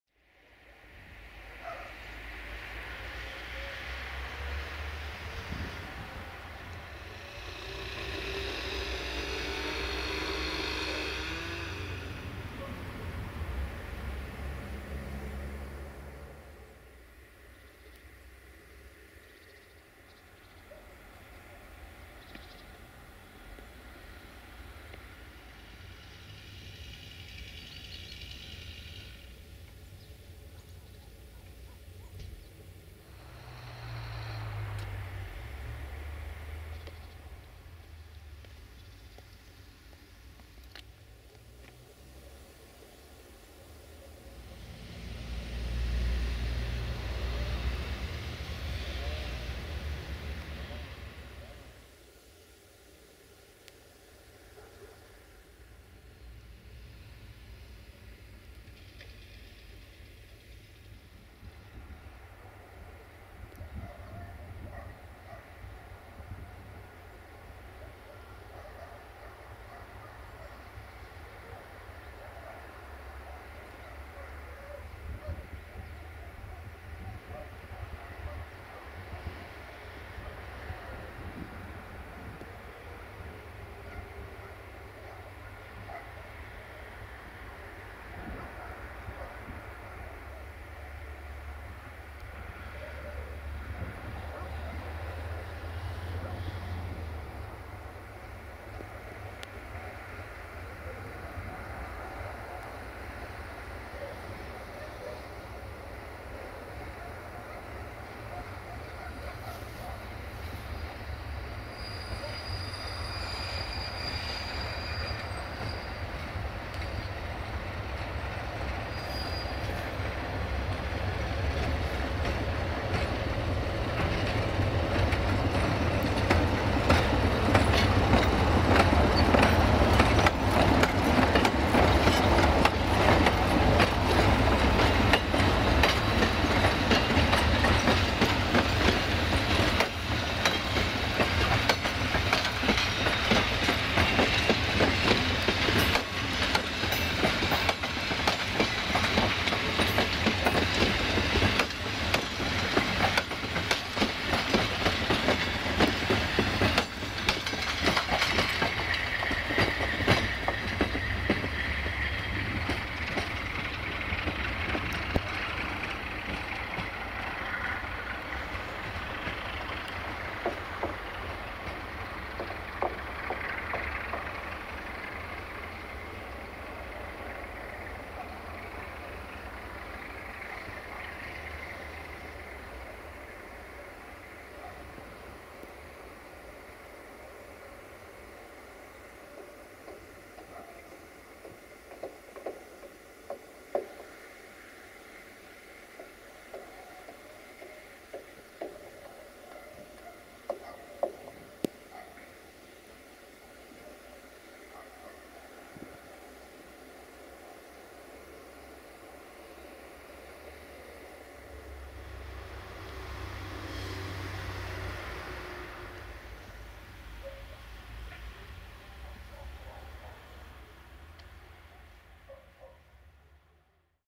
060-DA 60-1213-7 & R4337 Oradea-Halmeu in Gara Oradea Station [Winter Edition] - 07 January 2019
060-DA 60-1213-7 & R4337 Oradea-Halmeu in Gara Oradea Station [Winter Edition] - 07 January 2019

1:R4337 - Oradea - Halmeu - 060-DA/LDE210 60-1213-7 RO-SNTFC

07 January 2019